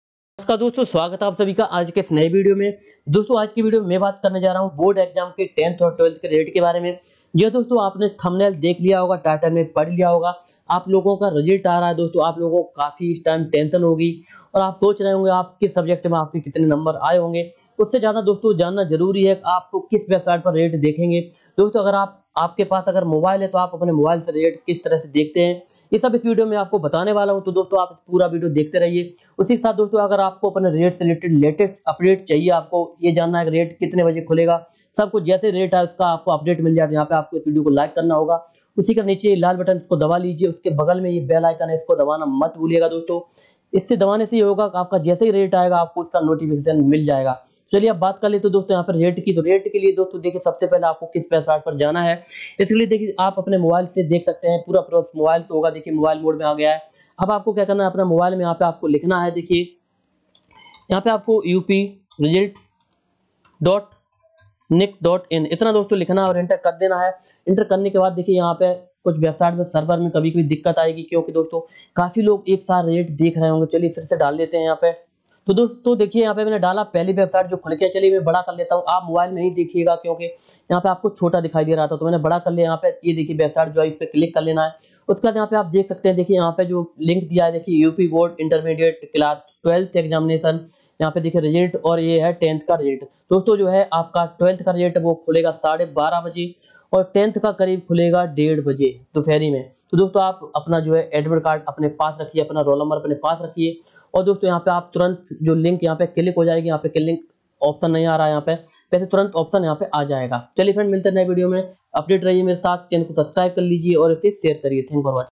10बी और 12बी रिजल्ट घोषित सबसे पहले कैसे देखे
aaj ke video me aapko baord exam result kr bare me bataya gya hai.